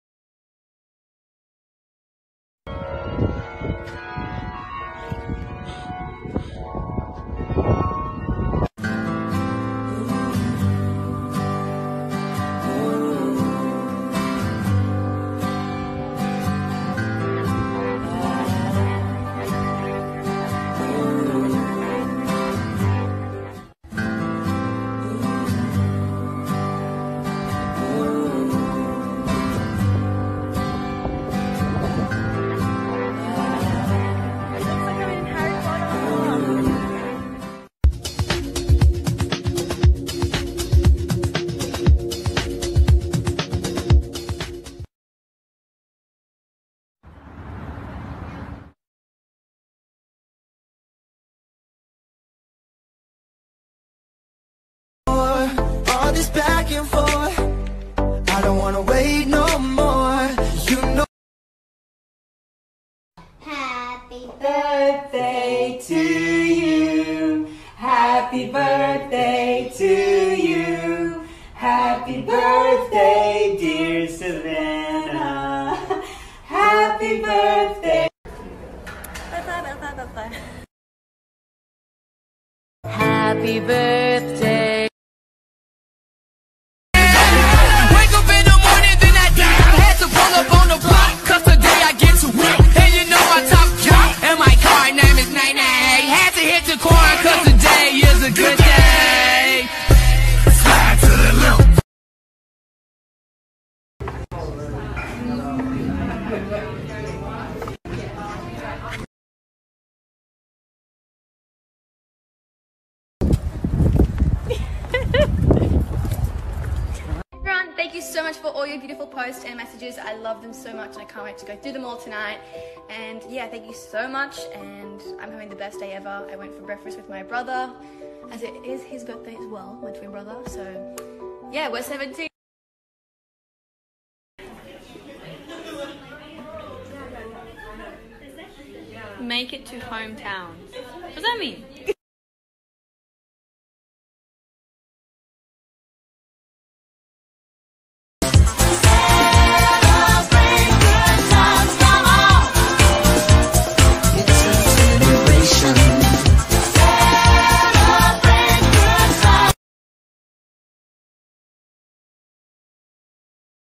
Savannah Clarke Stories (@savannah.clarke) - 05/7 a 11/7/2020
Este é um compilado de todos os stories que a dançarina e cantora do Now United, Savannah Clarke (@savannah.clarke) postou em seus stories no instagram durante a semana de 05/7/2020 a 11/7/2020. Pode conter stories retirados de outros perfis, tais como os de outros membros, familiares e amigos da mesma.